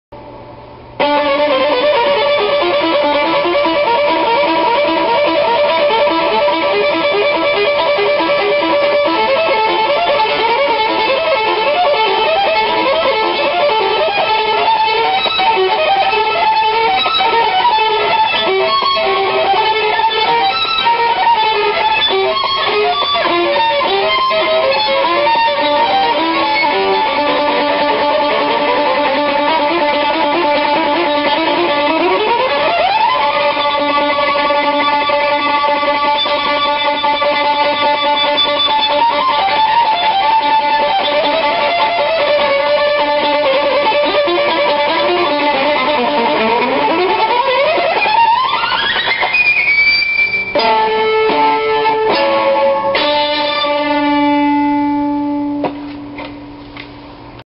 Novacek, Electric Fiddle
Just messing around with my friend's electric violin. Who said classical music doesn't sound modern?

And sorry to all those out there who can hear all my intonation faults :)